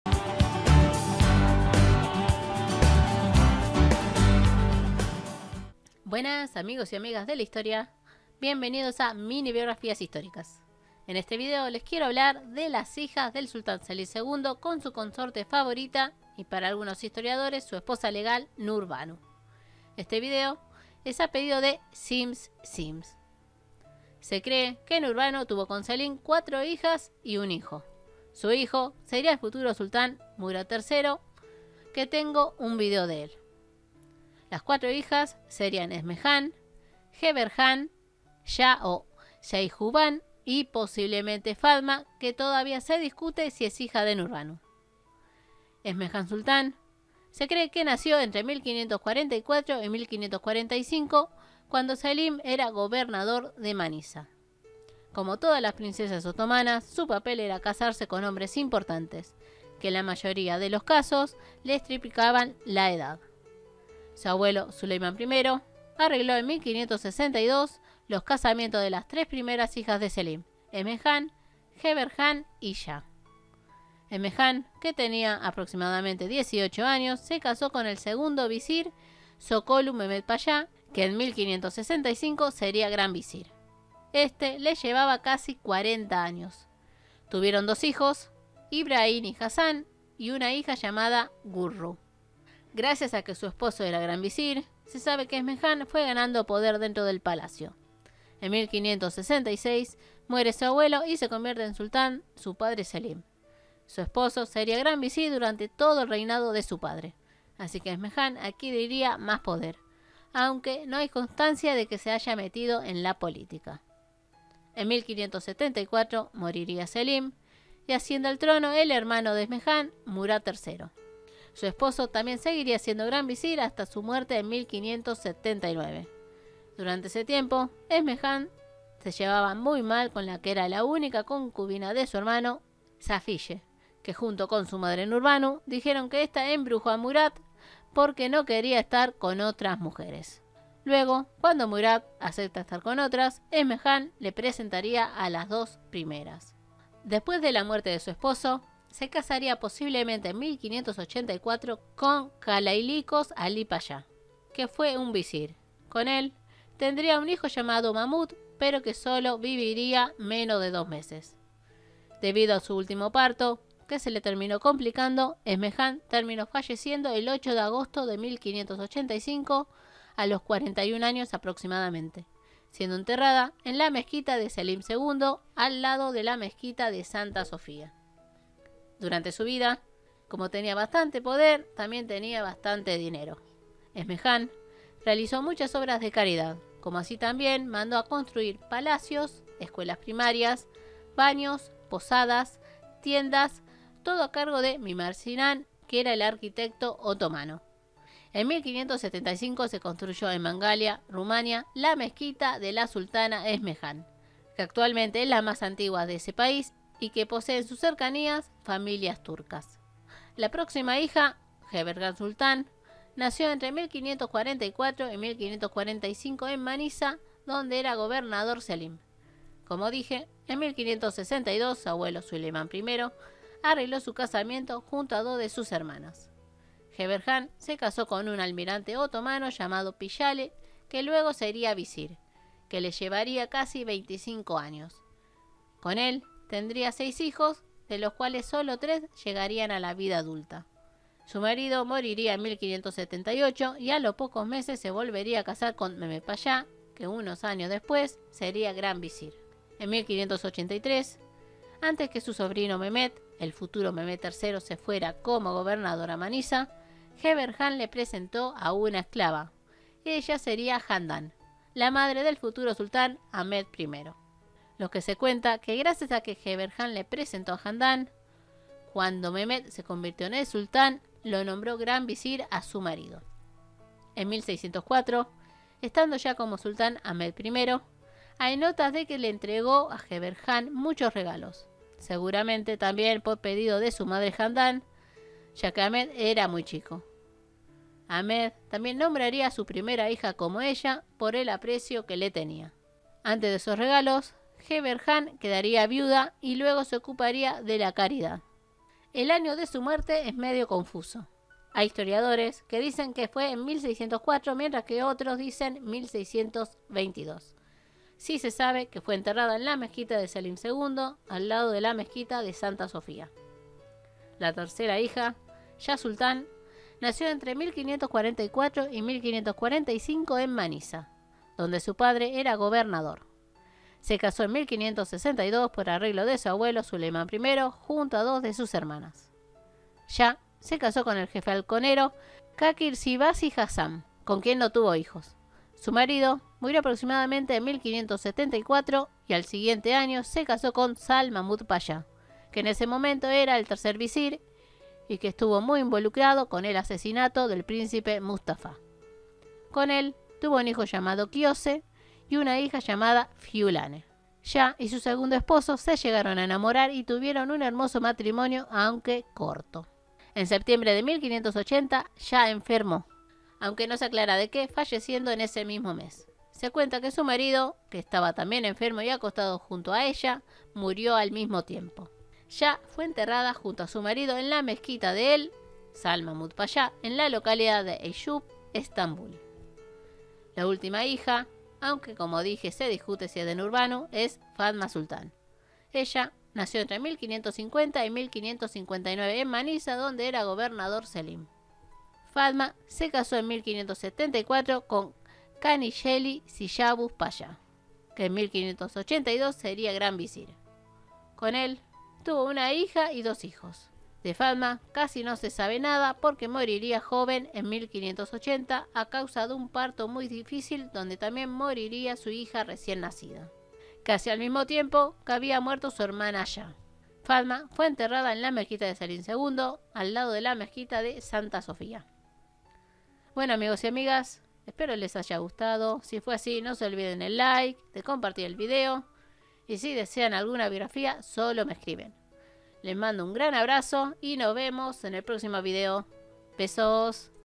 HIJAS de NURBANU y SELIM II | IMPERIO OTOMANO | EDAD MODERNA
🎁 NURBANU Y SELIM II tuvieron 4 HIJAS: Esmehan, Gevherhan, Sah y posiblemente Fatma.
Las imágenes de las princesas como de algunos maridos son de carácter ilustrativo.